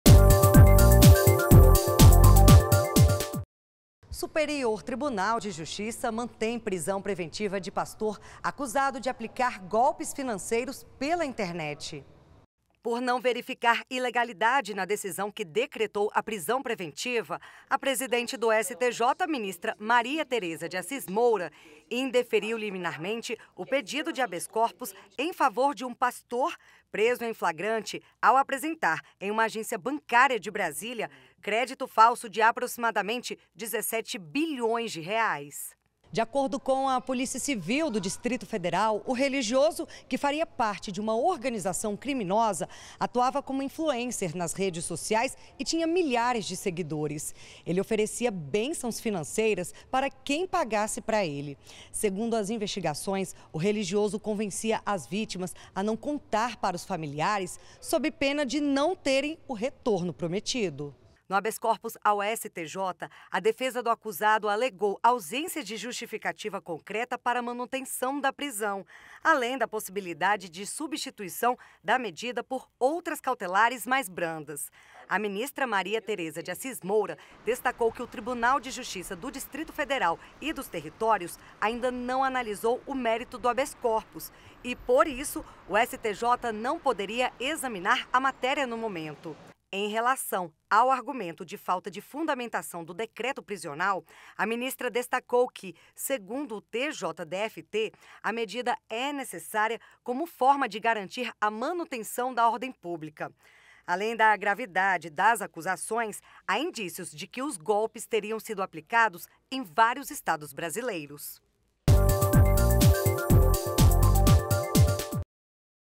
STJ mantém prisão preventiva de pastor acusado de aplicar golpes financeiros pela internet
​Por não verificar ilegalidade na decisão que decretou a prisão preventiva, a presidente do Superior Tribunal de Justiça (STJ), ministra Maria Thereza de Assis Moura, indeferiu liminarmente pedido de habeas corpus em favor de um pastor acusado de, por meio das redes sociais, prometer "bênçãos financeiras" após exigir que os seus seguidores realizassem investimentos em favor dele.

De acordo com a Polícia Civil do Distrito Federal, o religioso – que faria parte de uma organização criminosa – atuava como influencer nas redes sociais e tinha milhares de seguidores em seu canal no YouTube, no qual ele oferecia as "bênçãos" mediante o pagamento de valores. Segundo as investigações, o religioso convencia as vítimas a não mencionar os fatos aos familiares, sob pena de não terem o retorno prometido.

O pastor foi preso em flagrante ao apresentar, em uma agência bancária de Brasília, crédito falso de aproximadamente R$ 17 bilhões. A prisão foi posteriormente convertida em preventiva, com determinação do bloqueio de suas contas nas redes sociais, e mantida em decisão liminar pelo Tribunal de Justiça do Distrito Federal e dos Territórios (TJDFT).